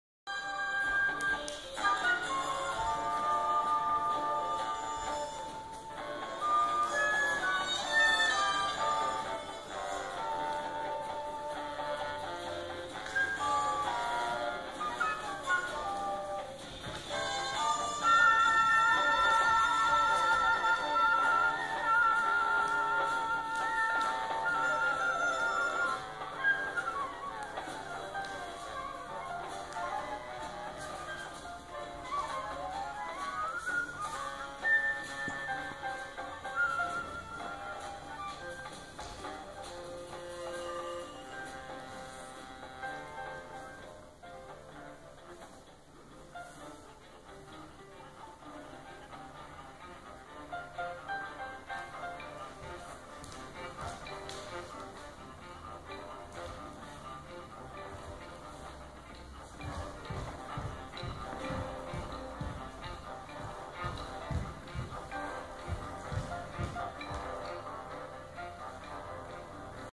Improvisation to music - Sun Ra
Chicago Dance/Movement Improvisation Gathering at Links Hall Studio A on 11/1/14.